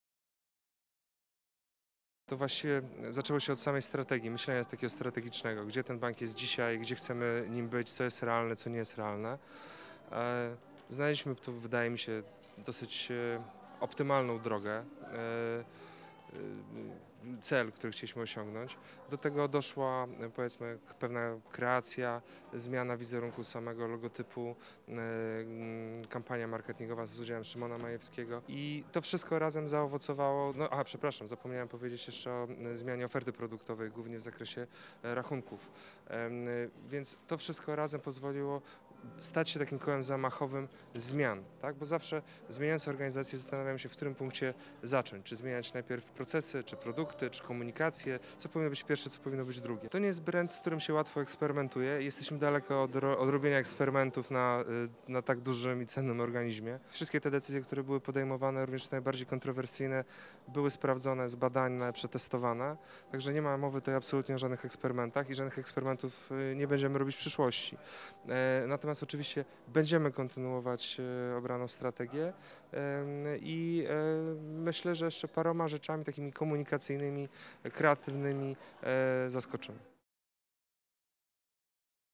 Strategia marketingowa zmieniła Bank PKO BP
Grand Prix Konkursu Dyrektor Marketingu Roku 2011 otrzymał Tomasz Marszałł (PKO BP).

Organizatorem wydarzenia jest spółka Mediarun, wydawca branżowych serwisów internetowych.